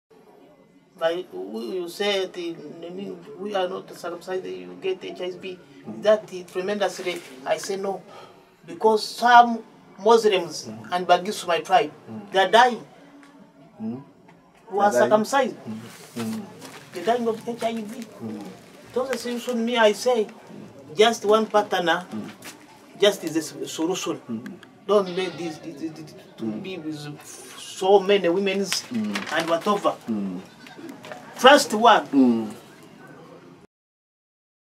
Trust One
Mass circumcision has failed to reduce HIV, so what is the solution? A Bagisu man offers his advice to young men.